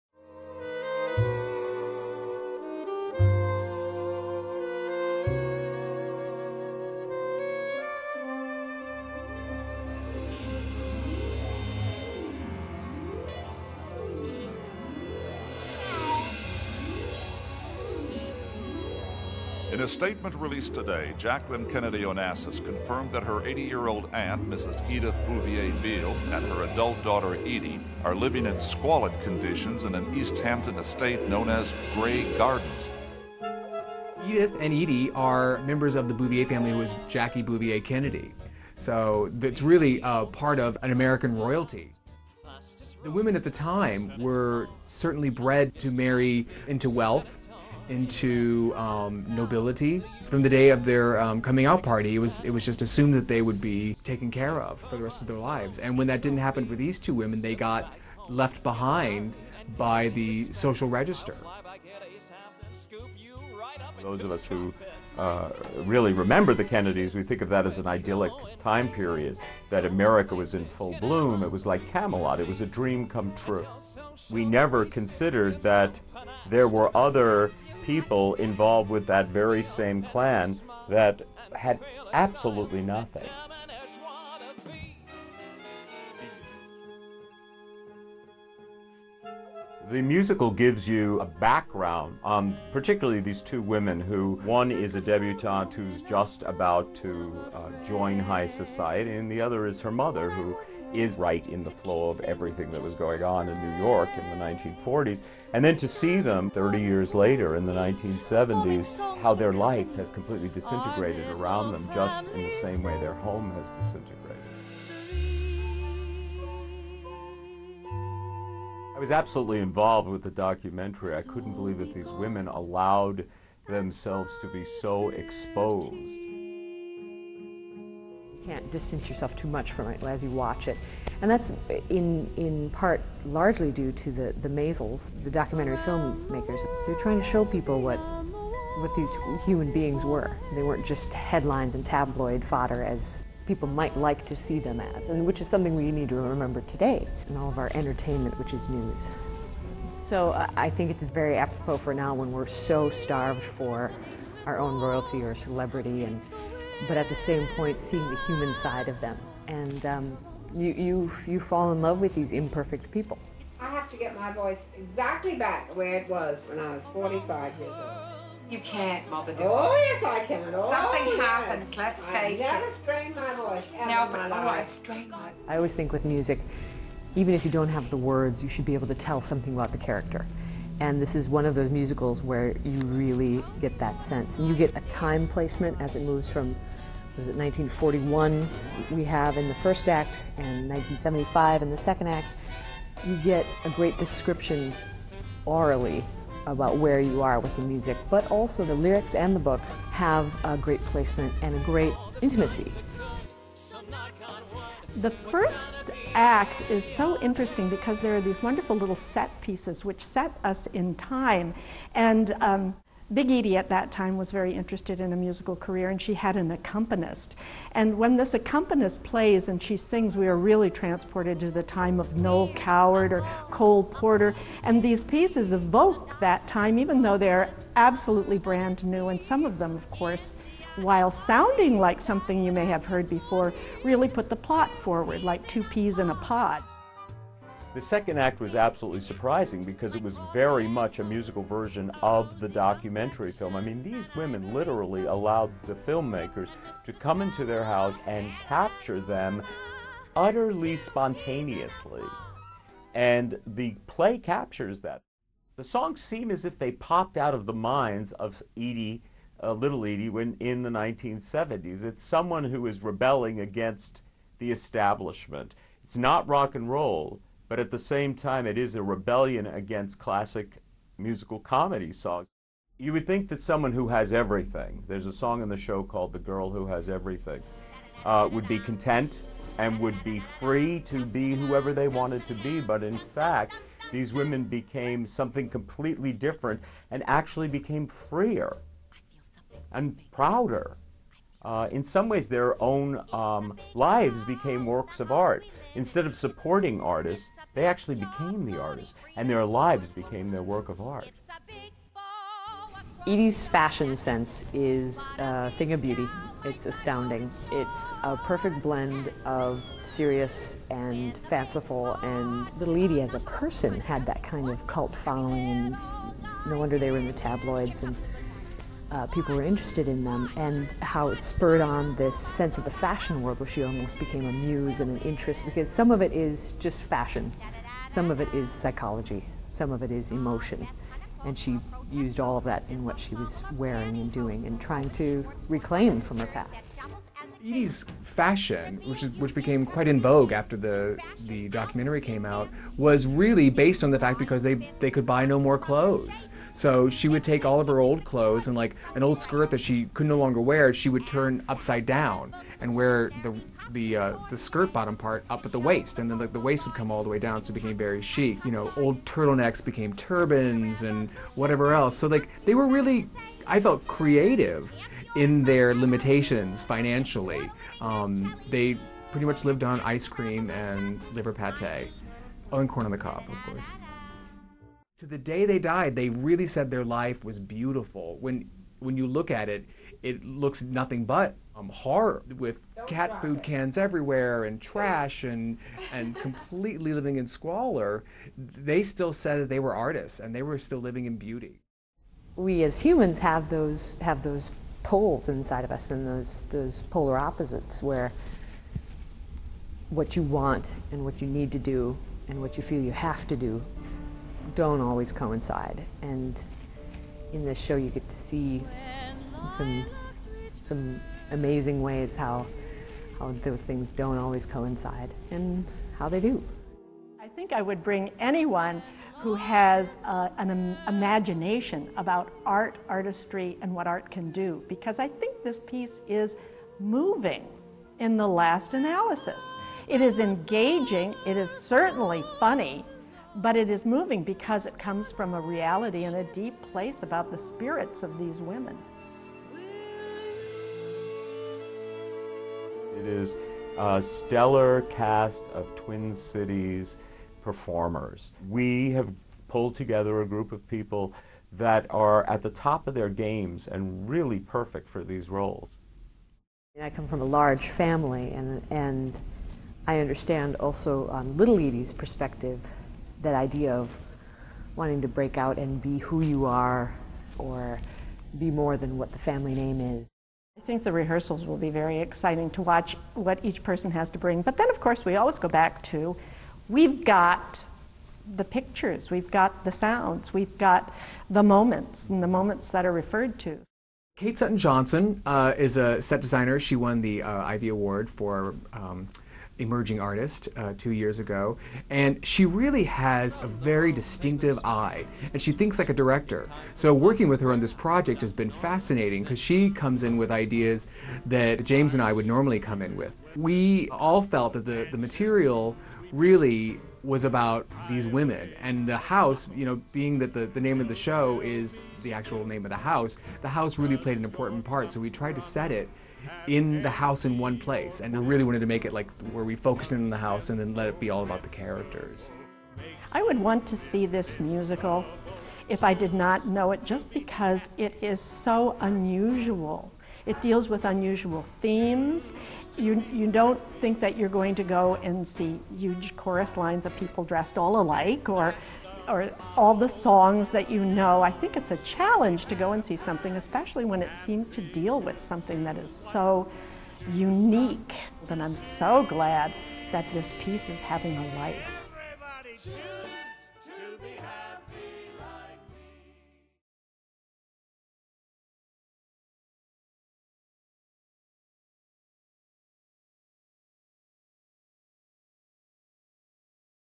A Look Inside Grey Gardens the Musical - Extended Cut
Take a look inside Ordway Center and Park Square Theatre's production of Grey Gardens The Musical with Extended Interviews with Co-Directors James Rocco and Jayme McDaniel, and leading cast members Christina Baldwin and Wendy Lehr.